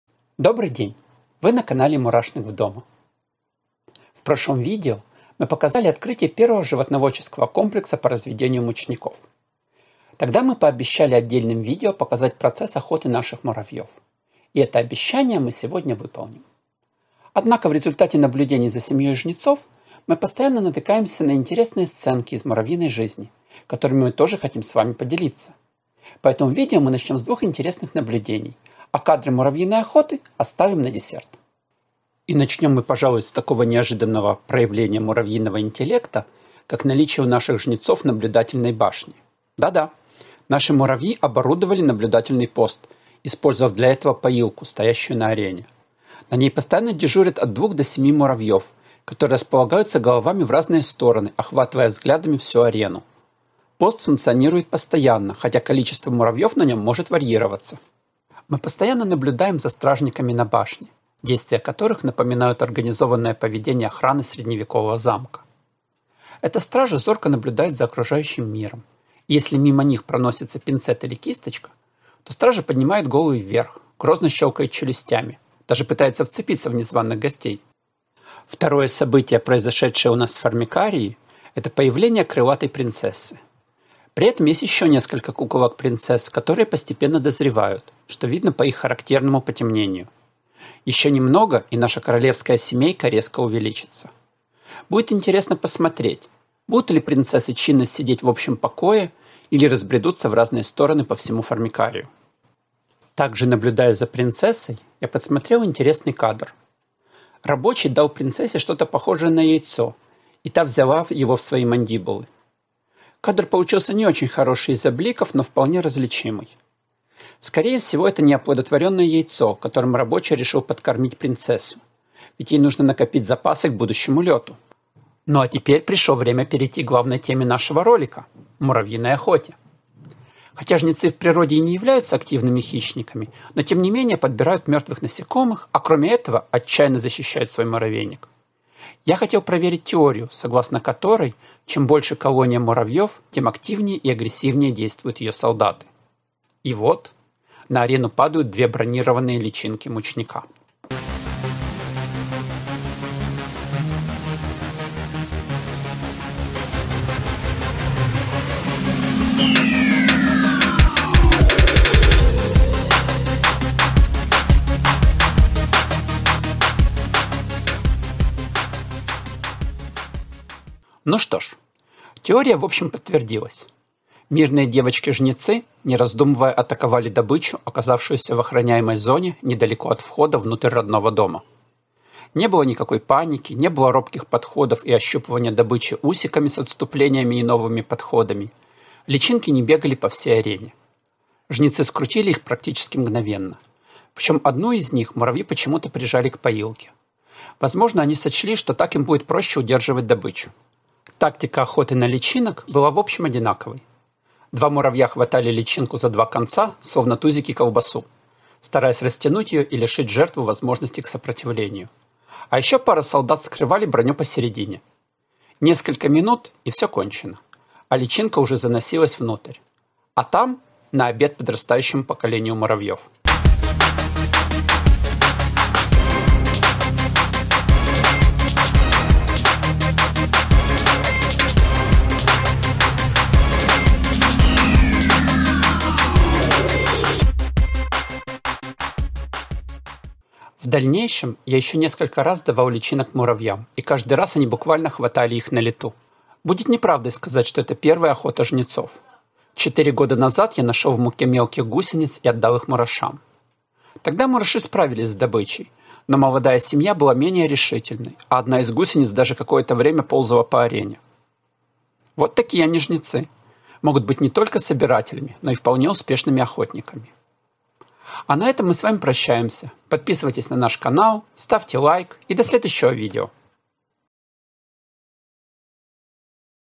Блог #09: Охота. Стражи на башне. Крылатая принцесса
1670-й день от Заселения семьи
Как мы и обещали в этом видео мы покажем охоту муравьев Messor Structor на личинок мучника, а также покажем крылатую принцессу и некоторые другие интересности.